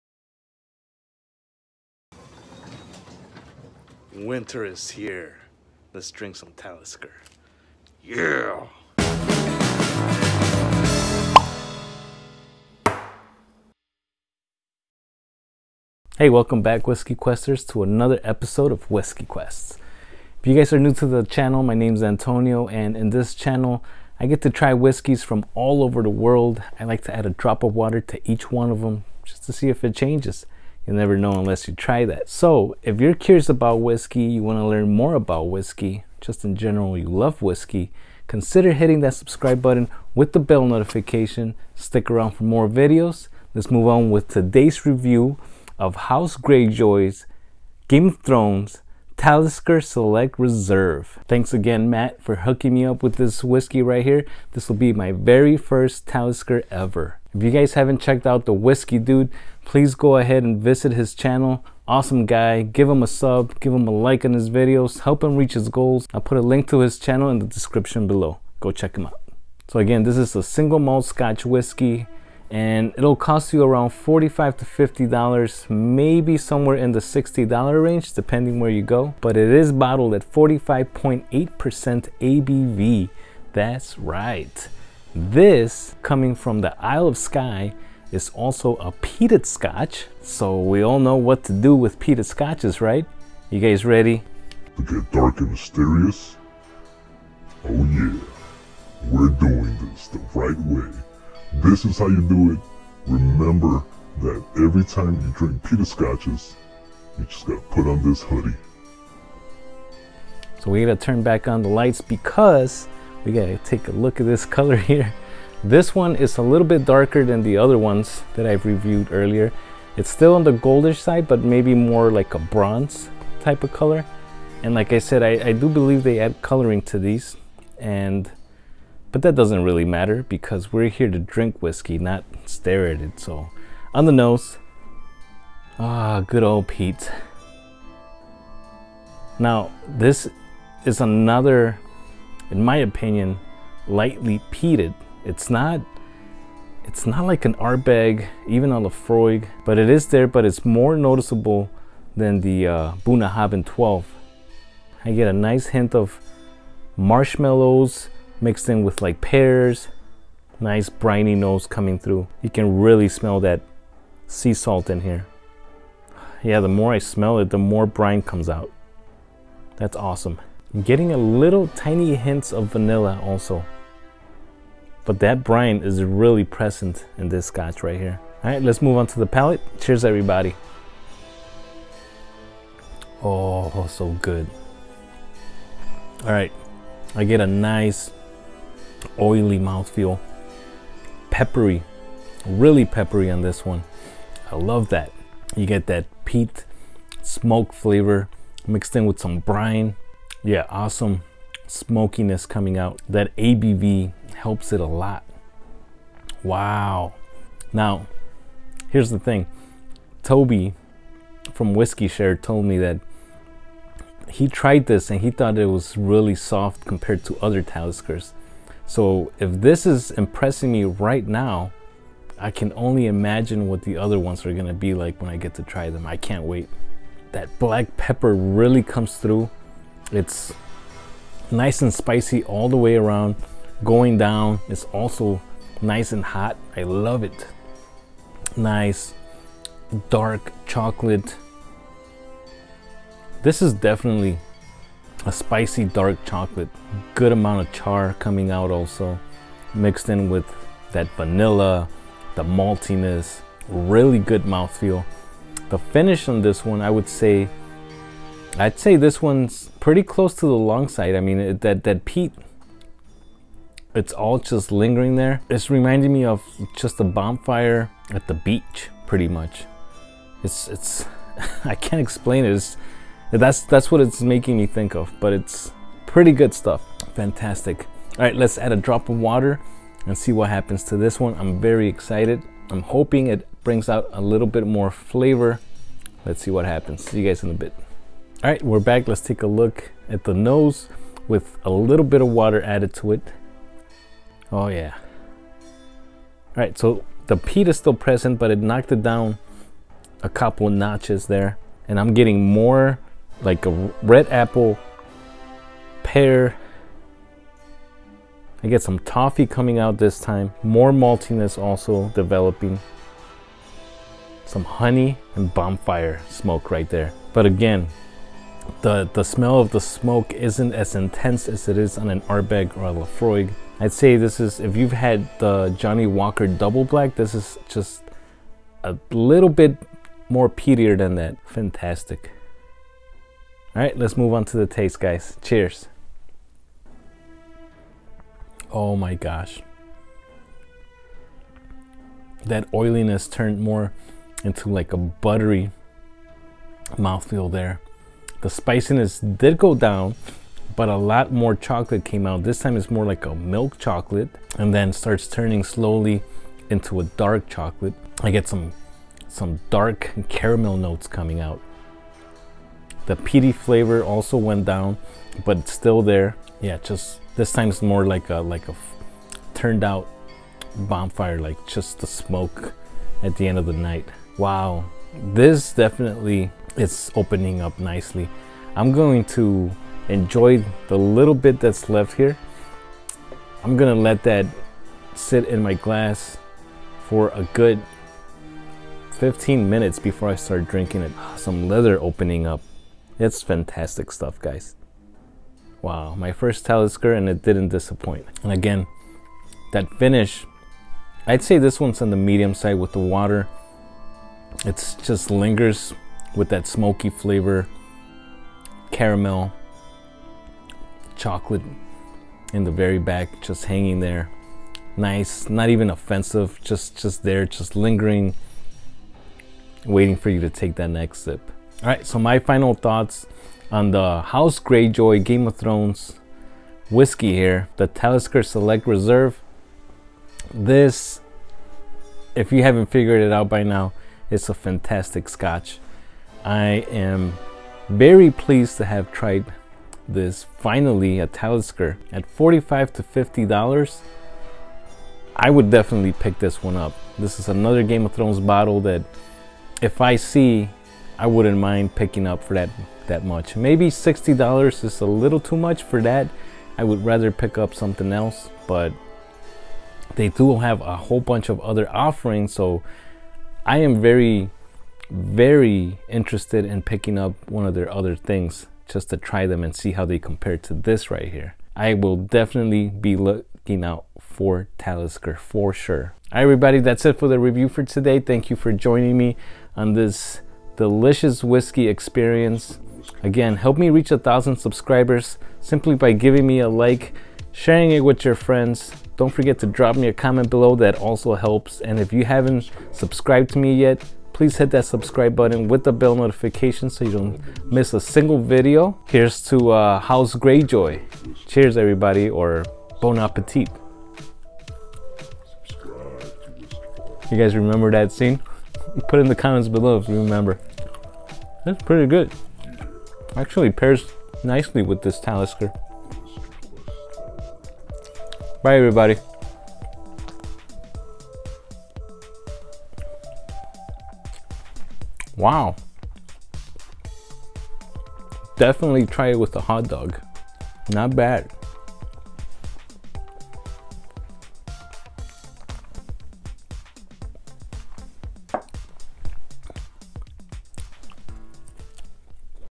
TASTING GAME OF THRONES TALISKER SELECT RESERVE - Isle of Skye Single Malt Scotch Whisky
This is a whisky review of the House Greyjoy Talisker Select Reserve Isle of Skye Single Malt Scotch Whisky. Bottled at 45.8% ABV peated scotch.  Limited release is it worth the hunt?

Thanks to the Whiskey Dude for donating these samples to the channel.

You Must be of Drinking Age to View This Content!

Please drink responsibly and don't drink and drive!